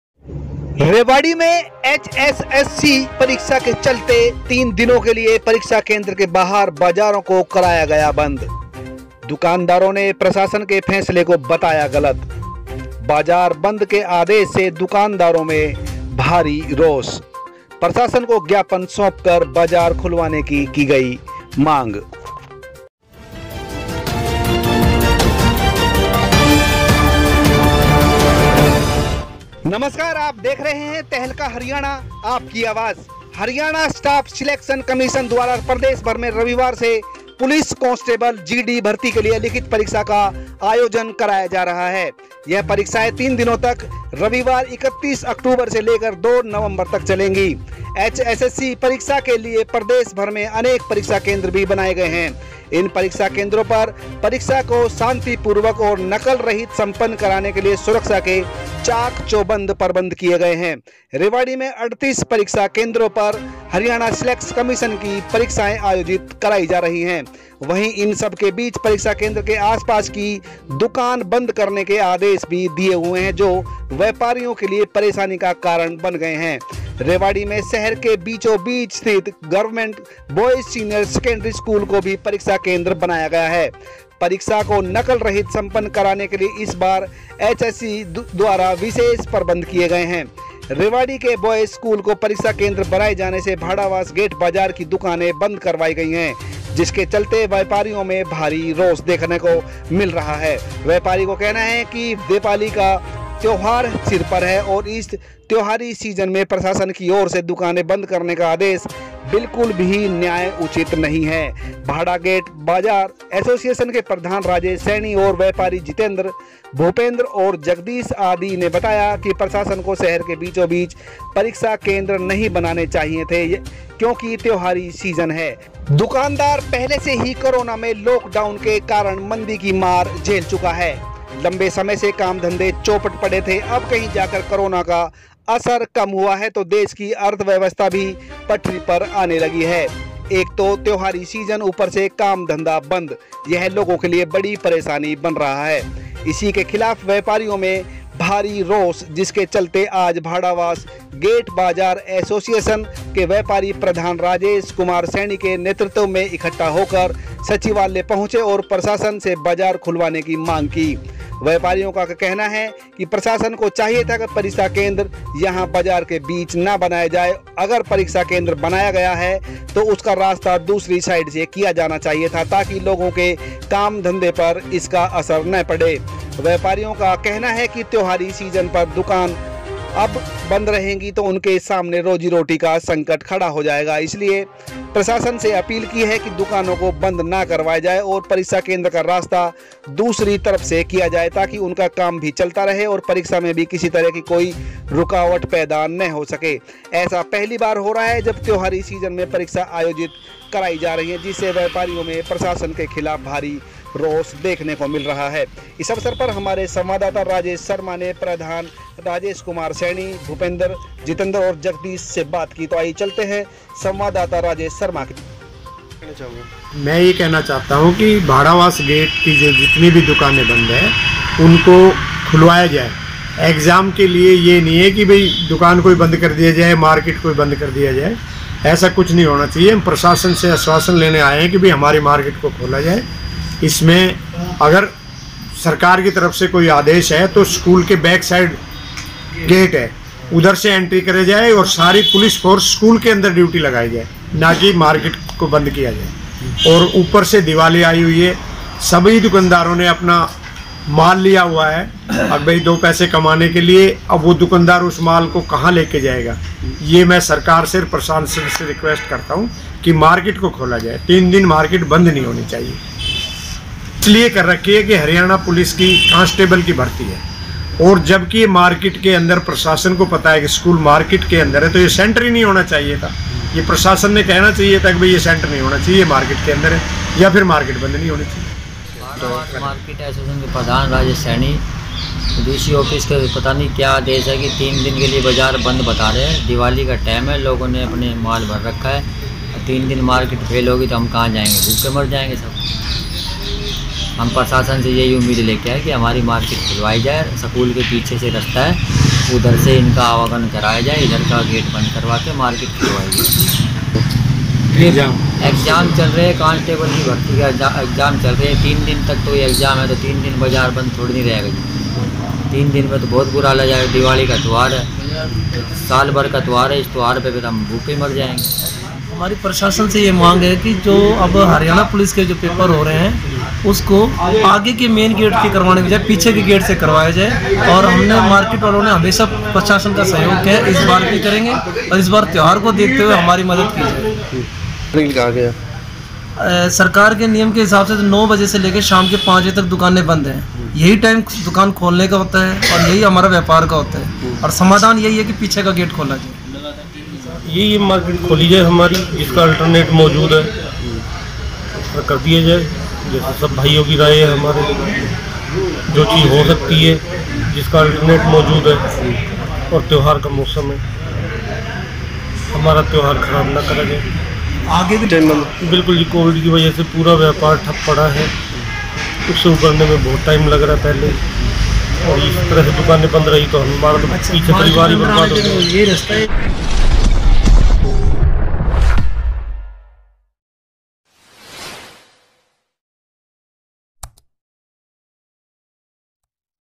रेवाड़ी में HSSC परिक्षा, बन्द कराई दुकानें, व्यापारी परेशान
रेवाड़ी में hssc परीक्षा के चलते 3 दिनों के लिए परीक्षा केंद्र के बाहर बाजार को करवाया गया बंद.
दुकानदारों ने प्रशासन के फैसले को बताया गलत.
बाजार बंद के आदेश से दुकानदारों में भारी रोष.
प्रशासन को ज्ञापन सौंपकर बाजार खुलवाने की मांग की.
एंकर : HSSC यानी हरियाणा स्टाफ सिलेक्शन कमीशन द्वारा प्रदेशभर में रविवार से पुलिस कॉन्स्टेबल GD भर्ती के लिए लिखित परीक्षा का आयोजन किया जा रहा है. यह परीक्षाएं तीन दिनों तक (रविवार 31 अक्टूबर से लेकर 2 नवंबर) तक चलेंगी. एचएससी परीक्षा के लिए प्रदेश भर में अनेक परीक्षा केंद्र भी बनाए गए हैं। इन परीक्षा केंद्रों पर परीक्षा को शांतिपूर्वक और नकल रहित संपन्न करवाने के लिए सुरक्षा के चाक-चौबंद प्रबंध किए गए हैं. रेवाड़ी में 38 परीक्षा केंद्रों पर एचएसएससी की परीक्षाएं आयोजित की जा रही है. वही इन सब के बीच परीक्षा केंद्र के आसपास की दुकान बंद करने के आदेश भी दिए हुए हैं जो व्यापारियों के लिए परेशानी का कारण बने हुए हैं. रेवाड़ी में शहर के बीचोंबीच स्थित गवर्नमेंट बॉयज सीनियर सेकेंडरी स्कूल को भी परीक्षा केंद्र बनाया गया है परीक्षा को नकल रहित संपन्न करवाने के लिए इस बार एचएससी द्वारा विशेष प्रबंध किए गए हैं.  रेवाड़ी के बॉयज स्कूल को परीक्षा केंद्र बनाए जाने से भाड़ावास गेट बाजार की दुकानें बंद करवाई गई है जिसके चलते व्यापारियों में भारी रोष है व्यापारियों का कहना है कि दिवाली का त्यौहार सिर पर है और इस त्योहारी सीजन में प्रशासन की ओर से दुकानें बंद करने का आदेश बिल्कुल भी न्याय उचित नहीं है. भाड़ावास गेट बाजार एसोसिएशन के प्रधान राजेश सैनी और व्यापारी जितेंद्र, भूपेंद्र और जगदीश आदि ने बताया कि प्रशासन को शहर के बीचो-बीच परीक्षा केंद्र नहीं बनाना चाहिए था क्योंकि त्योहारी सीजन है। दुकानदार पहले से ही कोरोना में लॉक डाउन के कारण मंदी की मार झेल चुका है। लंबे समय से काम धंधे चौपट पड़े थे अब कहीं जाकर कोरोना का असर कम हुआ है तो देश की अर्थव्यवस्था भी पटरी पर आने लगी है। एक तो त्योहारी सीजन ऊपर से काम-धंधा बंद यह लोगों के लिए बड़ी परेशानी बन रहा है. इसी के खिलाफ व्यापारियों में भारी रोष है जिसके चलते आज भाड़ावास गेट बाजार एसोसिएशन के व्यापारी प्रधान राजेश कुमार सैनी के नेतृत्व में इकट्ठा होकर सचिवालय पहुंचे और प्रशासन से बाजार खुलवाने की मांग की। व्यापारियों का कहना है कि प्रशासन को चाहिए था कि परीक्षा केंद्र यहां बाजार के बीच ना बनाया जाए अगर परीक्षा केंद्र बनाया गया है तो उसका रास्ता दूसरी साइड से किया जाना चाहिए था ताकि लोगों के काम धंधे पर इसका असर ना पड़े. व्यापारियों का कहना है कि  त्योहारी सीजन पर अगर दुकानें बंद रहेगी तो उनके सामने रोजी रोटी का संकट खड़ा हो जाएगा. इसलिए प्रशासन से अपील है की दुकानों को बंद ना करवाया जाए और परीक्षा केंद्र का रास्ता दूसरी तरफ से किया जाए ताकि उनका काम भी चलता रहे और परीक्षा में भी किसी तरह की कोई रुकावट पैदा ना हो.  ऐसा पहली बार हो रहा है जब त्योहारी सीजन में परीक्षा आयोजित की जा रही है.जिससे व्यापारियों में प्रशासन के खिलाफ भारी रोष है।
 बाइट : राजेश कुमार सैनी : प्रधान भाड़ावास गेट मार्केट. बाइट : भूपेंद्र, जितेंद्र और जगदीश दुकानदार.(@tahalkaharyana)